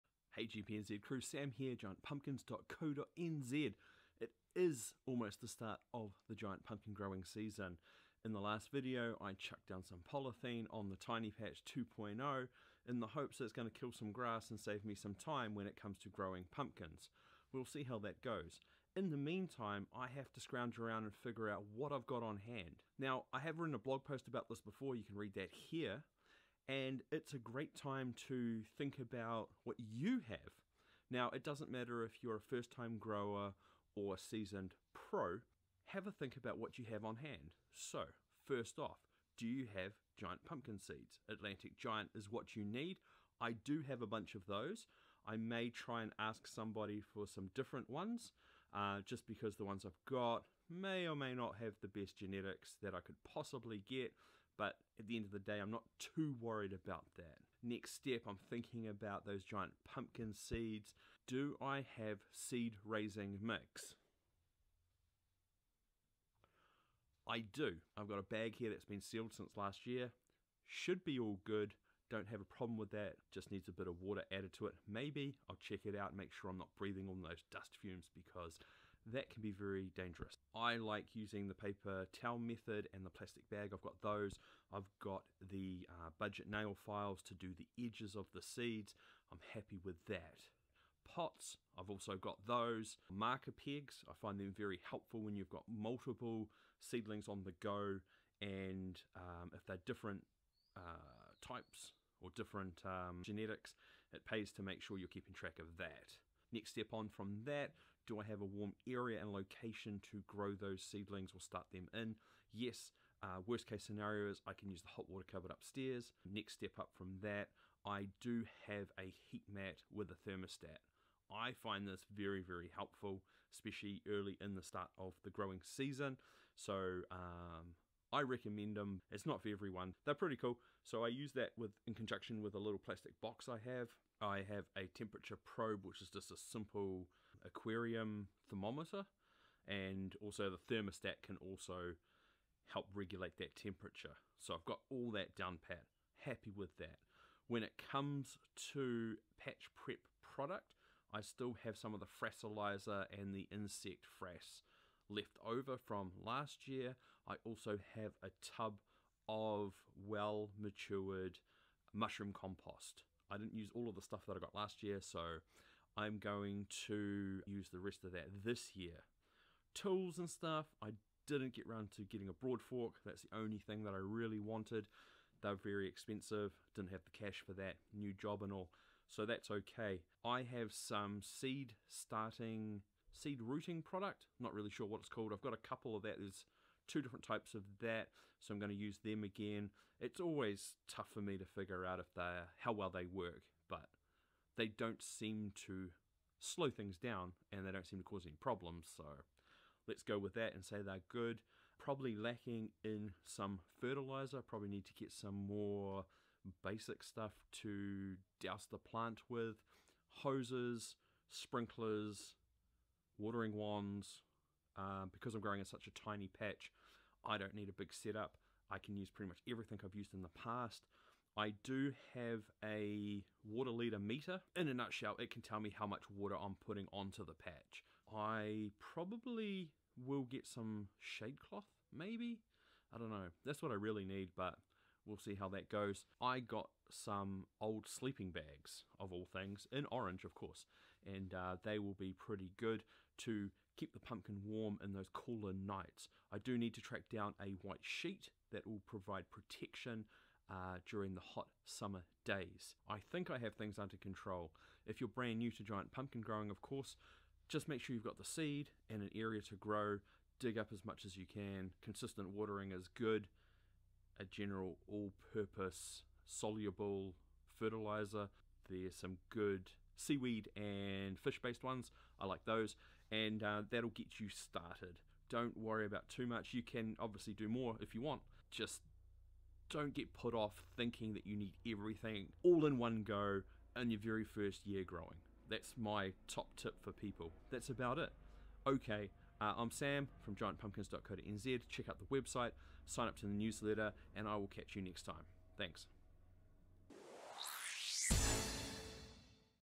Figuring out what I have on hand for my Giant Pumpkin Growing Season
The start of the giant pumpkin growing season means finding out what I have on hand and what things I may still need to sort out. Come along as I work out what I have.